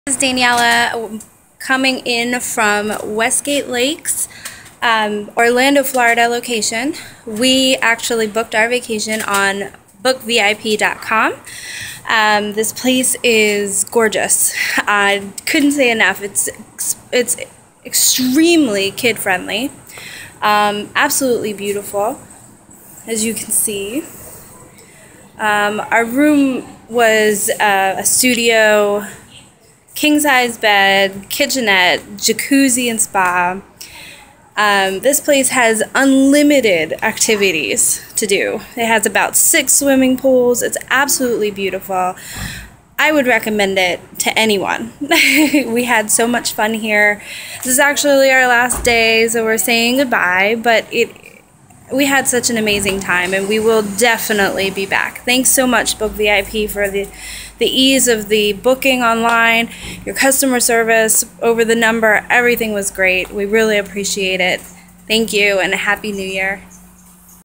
BookVip.com Customer Review of Westgate Lakes Resort Orlando
A BookVip.com gives a video review of their vacation experience at Westgate Lakes Resort in Orlando Florida